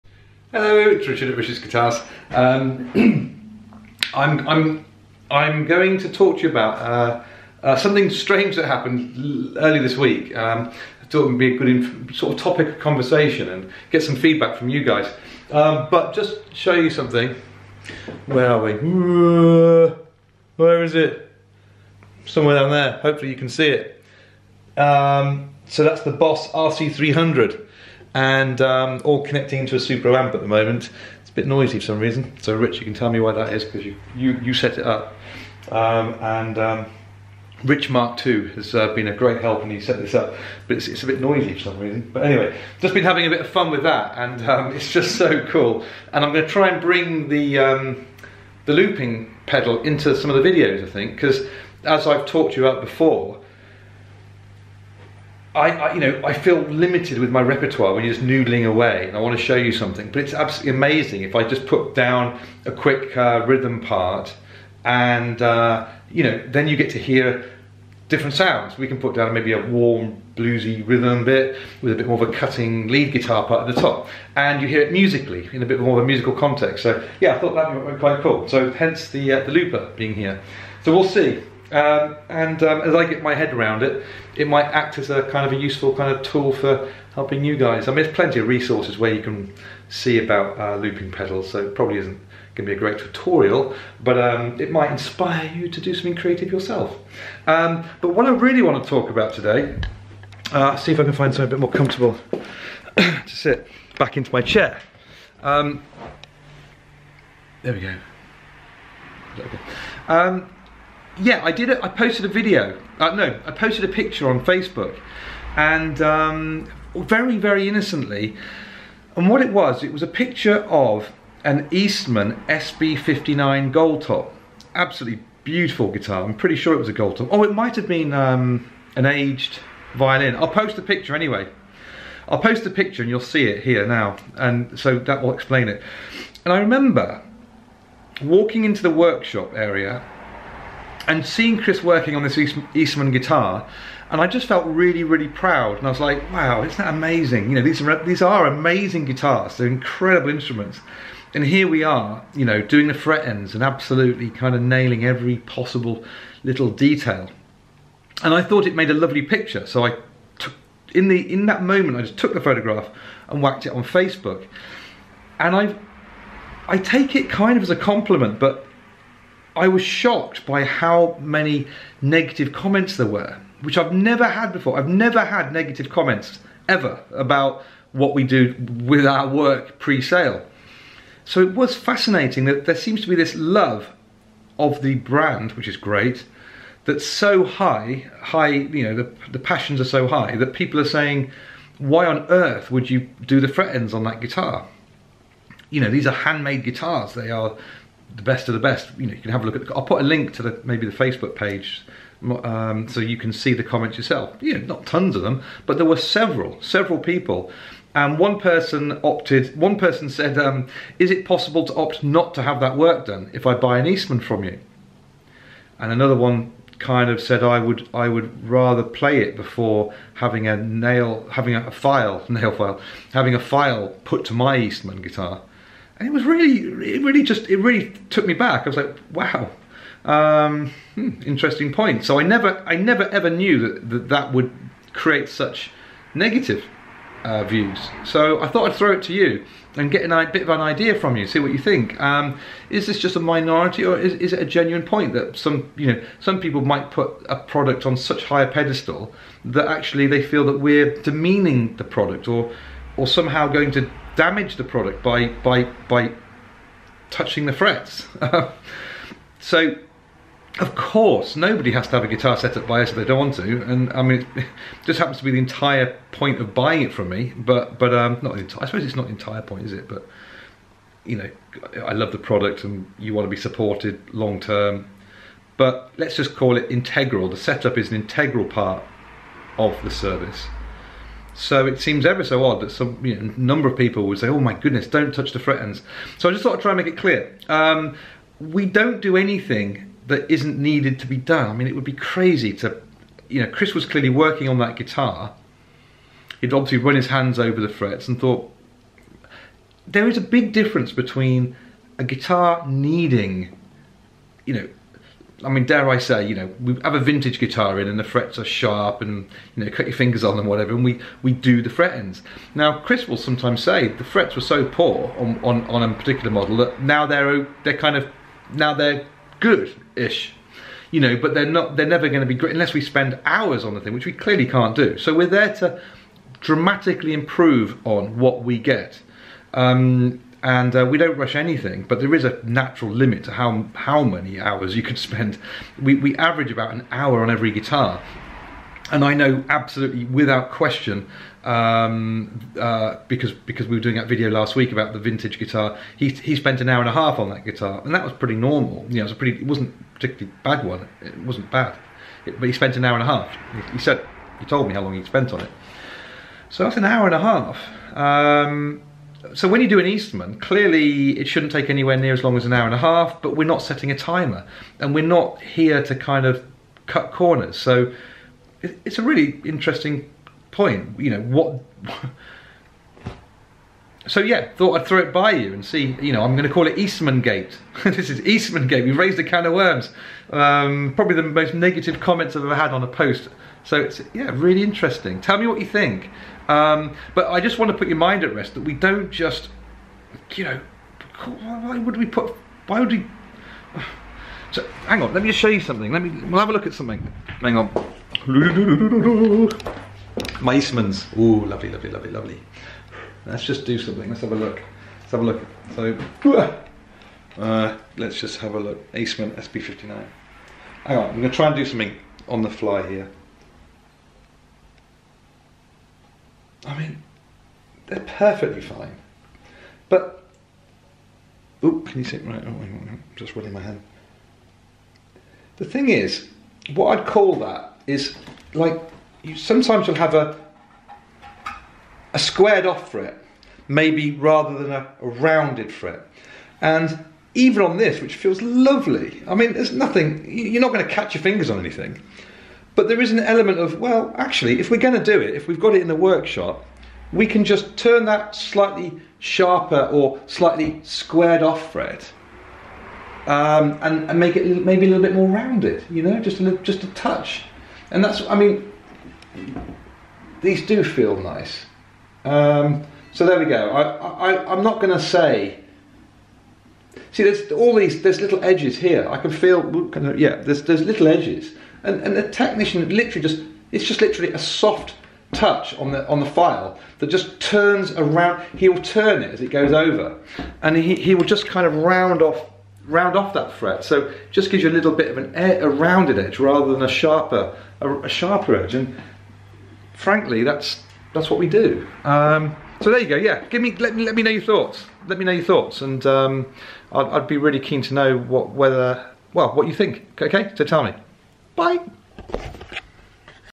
"Eastmangate" - Facebook Pic Of Eastman Being Setup Causes Outrage!
Last week I posted a pic that I took literally "in the moment".  It was actually a moment of pride seeing this wonderful Eastman SB59 on the workshop bench being lovingly setup prior to dispatch.

However. The photo really did seem to cause offense which I could never have forseen - nearly EVERY post questioning our need to do the fret ends - rather than praising us for our commitment to perfection.

It does raise an interesting point though.  At what point does our "intervention" become unwanted?!

One Facebook comment asked if they could request to not have the work done on their guitar!  A stab to the heart - but I have to listen to this kind of comment - hence asking you what your thoughts are.

Its a funny old world but hand on heart - sorry if the photo scared anyone.

EVERY guitar we EVER sell goes through a process.  It does not mean the guitar is dreadful or the guitar is faulty.  Its just our way of ensuring it is as good as every other guitar we have done for the past 24 years!